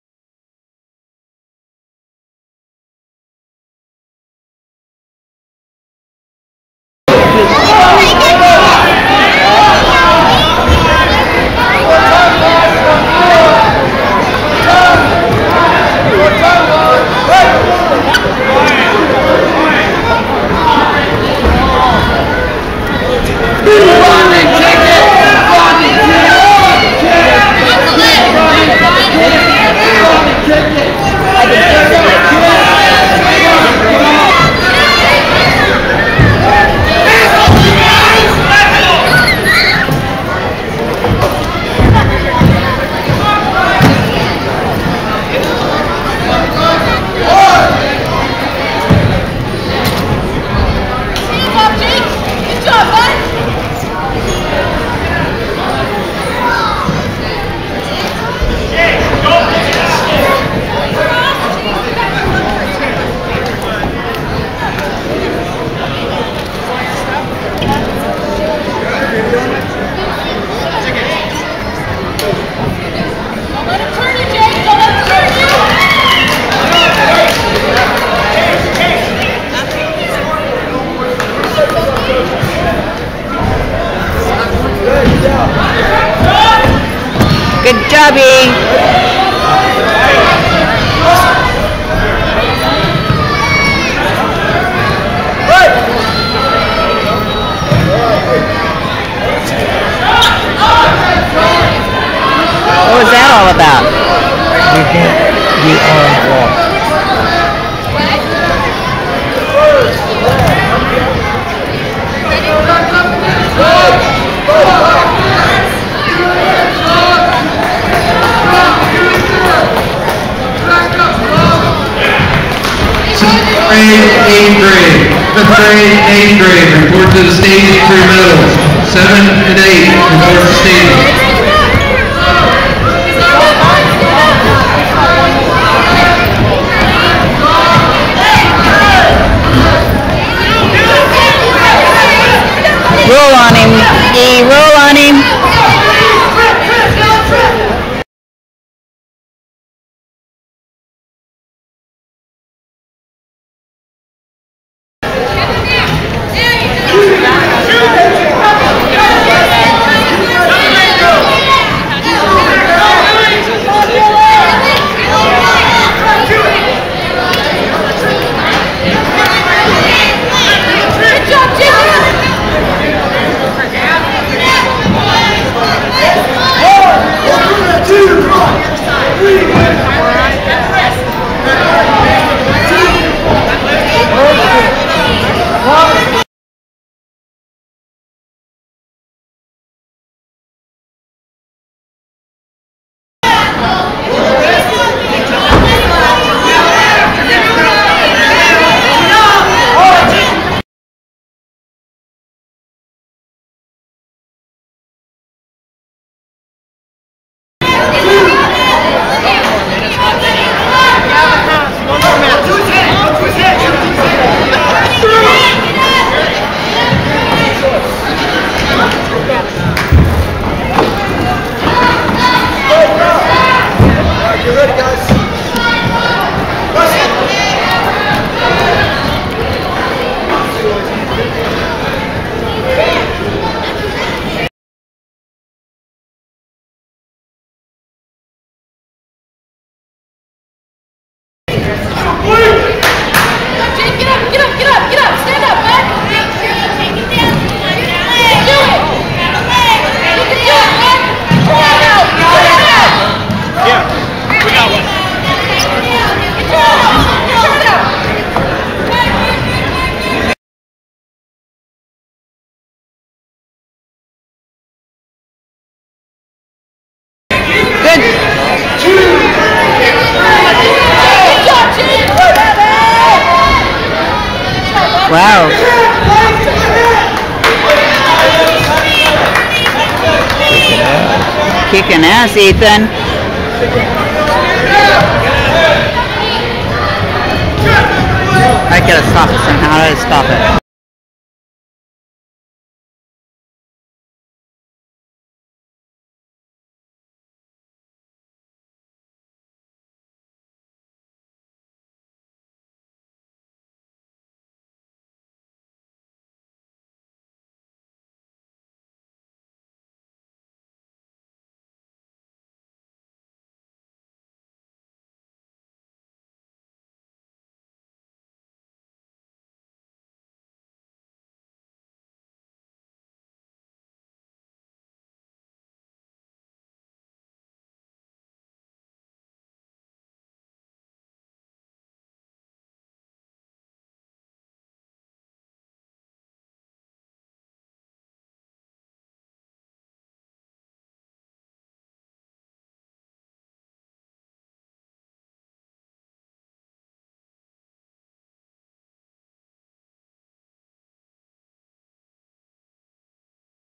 Ethan's tournament. Dec 17 Harrisburg, OR..
Last match of the tournament!  Ethan took third in his class!